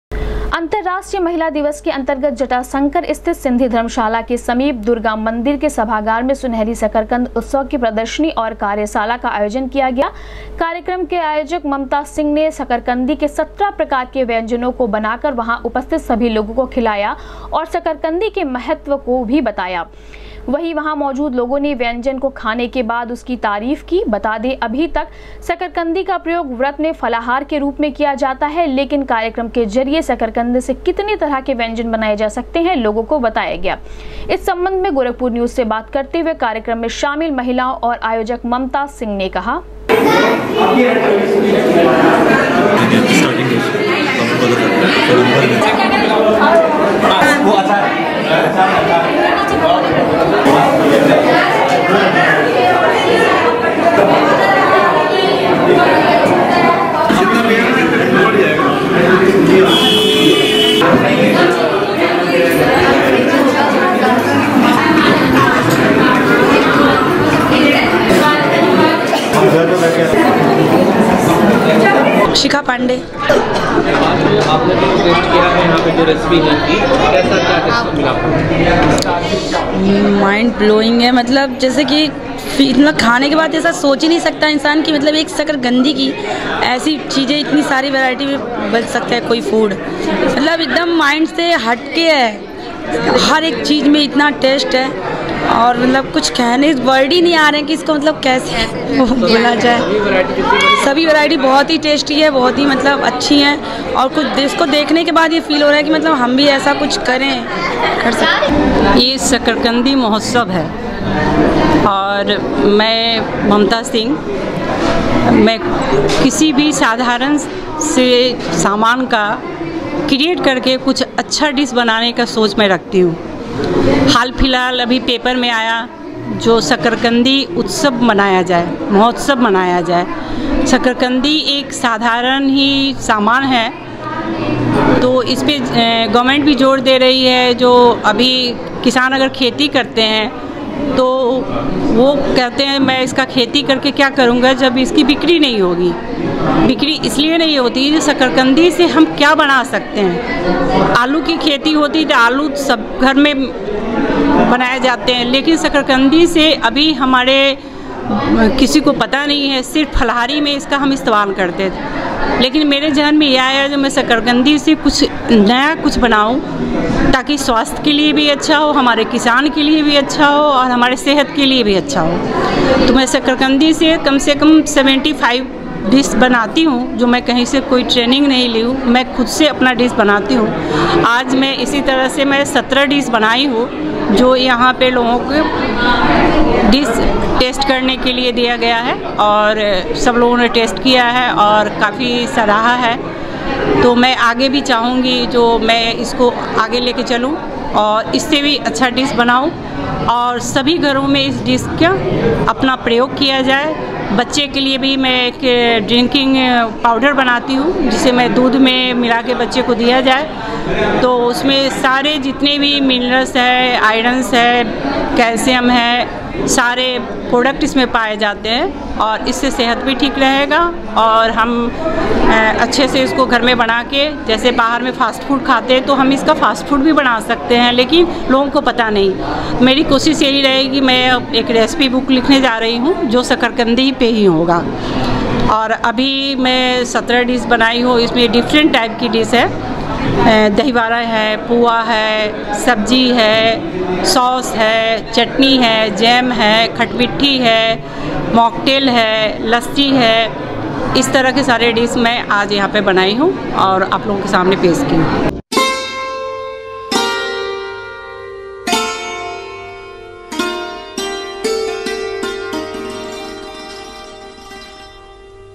सुनहरी शकरकंदी उत्सव की प्रदर्शनी और कार्यशाला का किया गया आयोजन,शकरकंदी से बने 17 प्रकार के व्यंजनो
सुनहरी शकरकंदी उत्सव की प्रदर्शनी और कार्यशाला का किया गया आयोजन, शकरकंदी से बने 17 प्रकार के व्यंजनों का लोगों ने चखा स्वाद